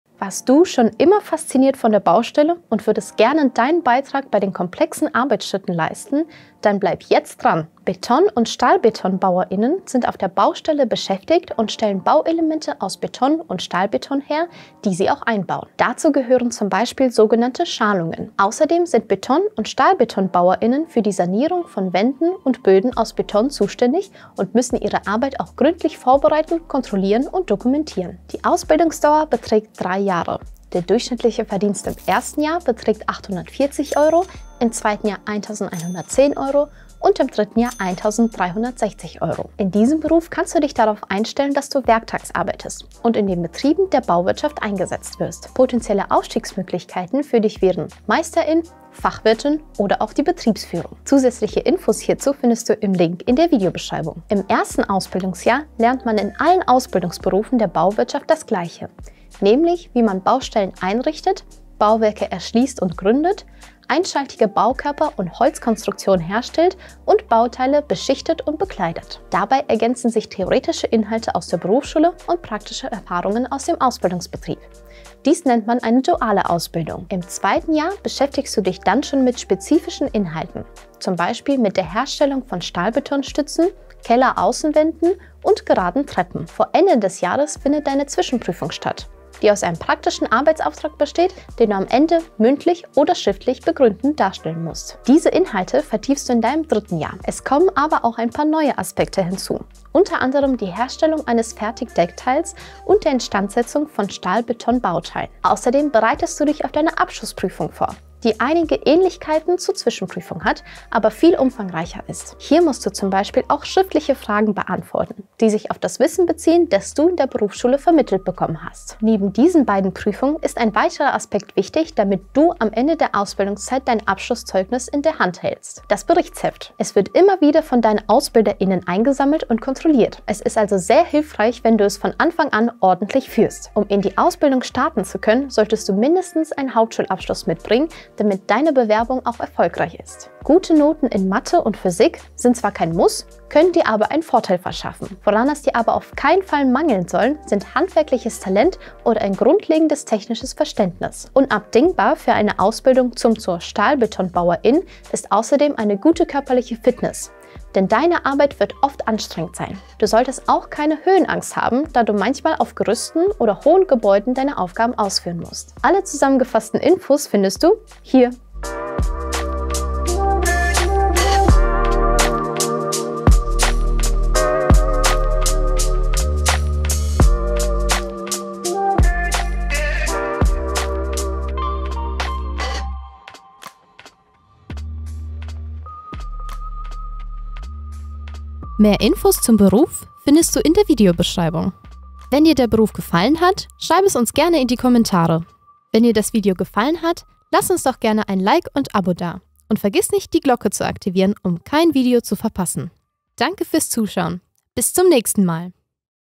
Beton- und Stahlbetonbauer:in - Ausbildungsberufe erklärt
In diesem Video erfährst du, wie die Rahmenbedingungen für den:die Beton- und Stahlbetonbauer:in sind. Du erfährst alle wichtigen Details rund um den Ablauf, Gehalt und wer für diesen Beruf geeignet ist. Du hast weitere Fragen zu dieser Ausbildung? Dann hinterlasse doch ruhig einen Kommentar!

Du interessierst dich für die Thematik Ausbildung und möchtest mehr über Berufsbilder erfahren und sogar deinen Traumjob finden?

00:00 Beton- und Stahlbetonbauer:in
00:08 Aufgabenbereich
00:28 Allgemeine Fakten
00:54 Ausbildungsablauf
02:14 Berufseignung
02:49 Zusammenfassung